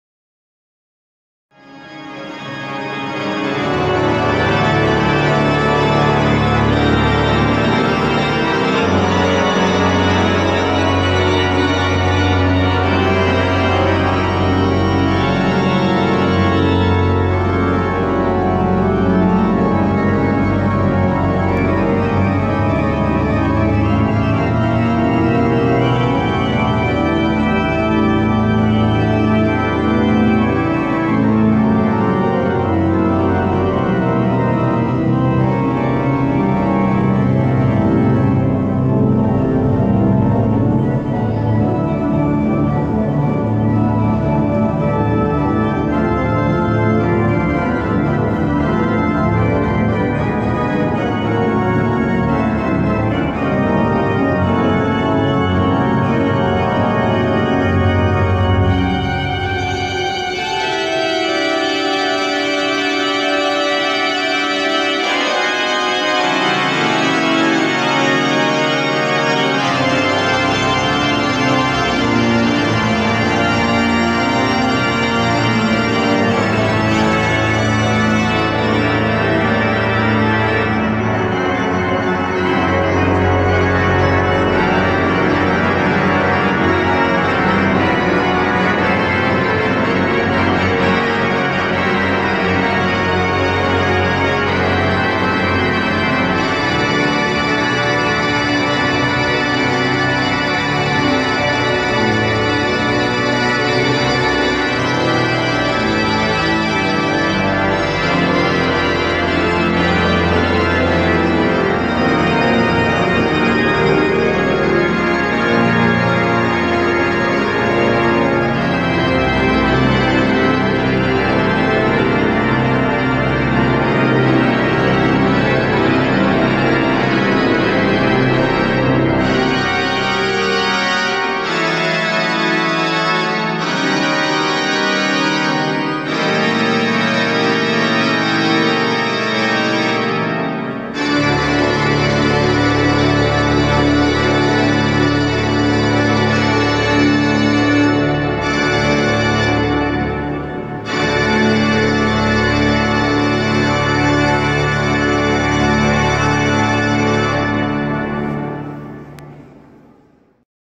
CHURCH ORGAN POWER by Graal7
cathédrale de valence
puissance des grandes orgues
messe de minuit 2019.